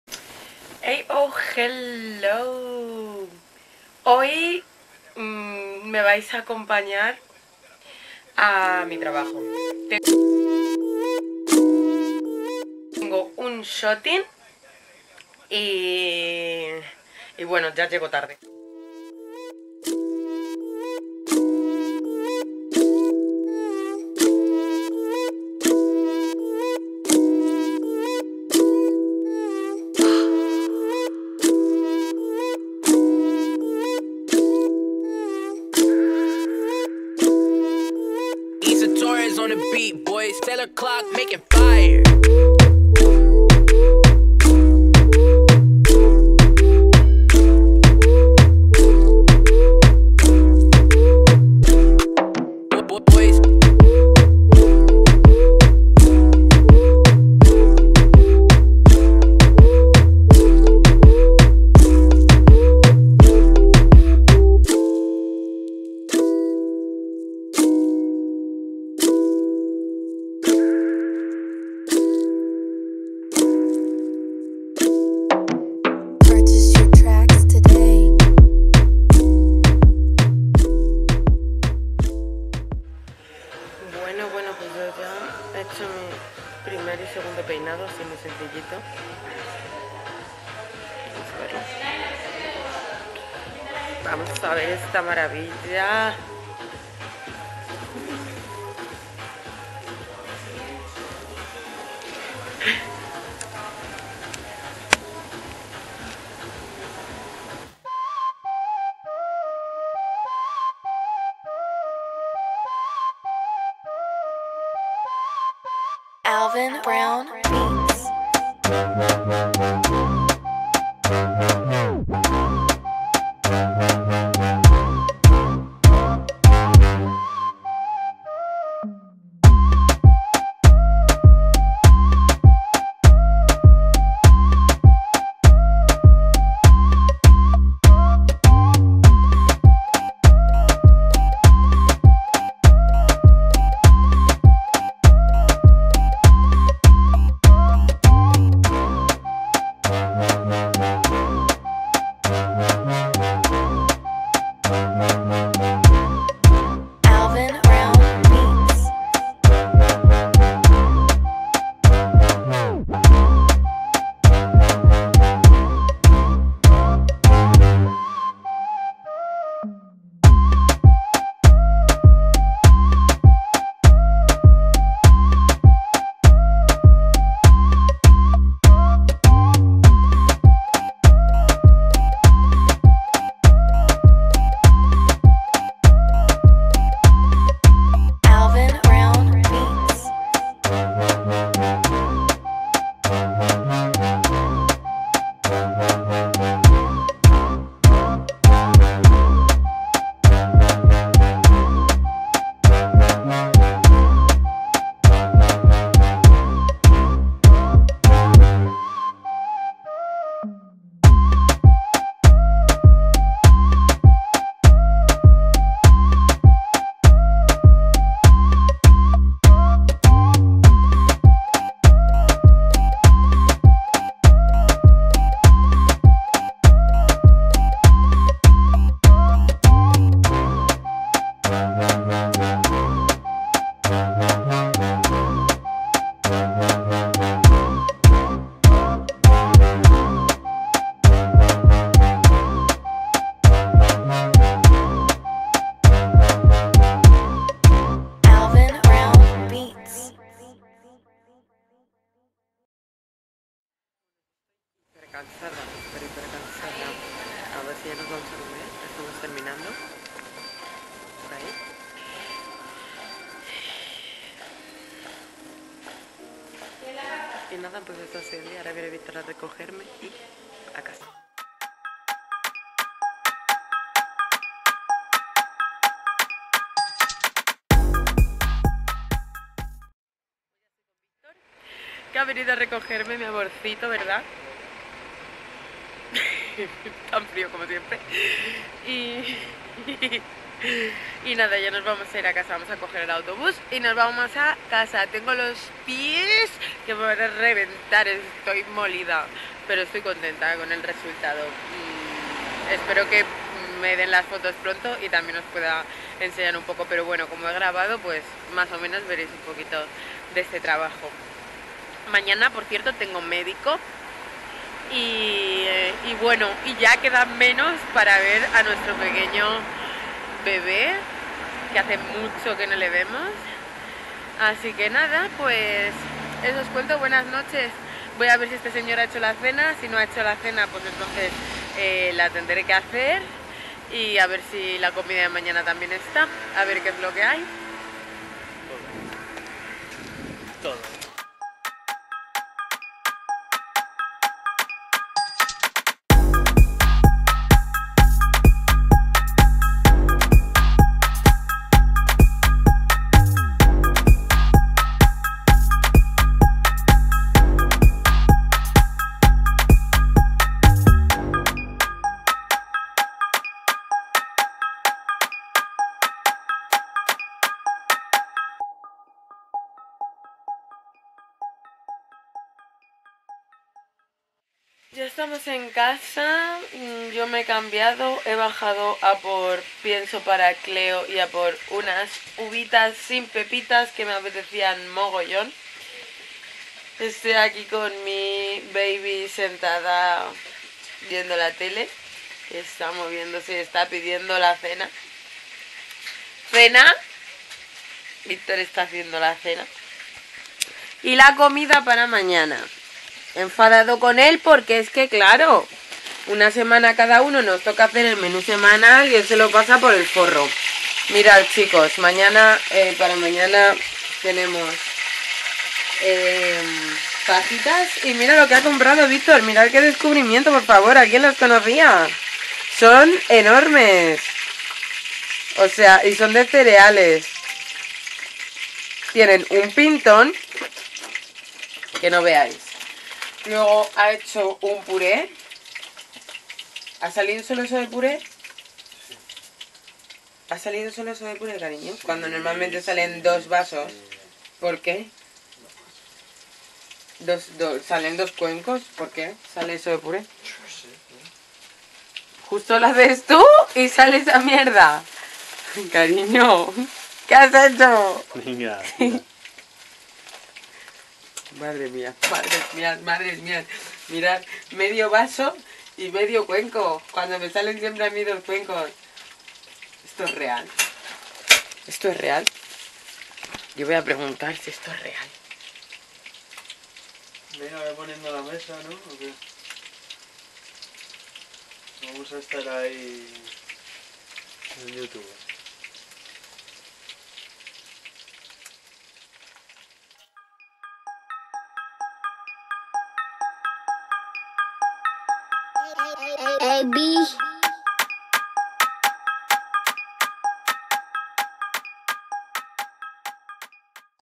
UN DIA EN MI TRABAJO| SHOOTING LA ZOWI Y ALBANY wear by BluettothGirl| Celebrity HairArtist
Hola mis Ghetto Babys!! Bienvenidos una vez más a mi canal!!
Hoy os traigo un video super especial. Me vais a acompañar a mi trabajo.
Las celebritys a las que peino son dos cantantes empoderadas que están marcando un hito en la música. La Zowi y Albany.
Además quiero que veais que aunque esté embaraza sigo manteniendo mi rutina de trabajo , un poquito más cansada pero con mucha energía. Este bebé que llevo dentro hace que me sienta la mujer más fuerte y empoderada del mundo!! Adoro esta sensación y adoro cada momento que nuestro pequeño pasa dentro de mi vientre.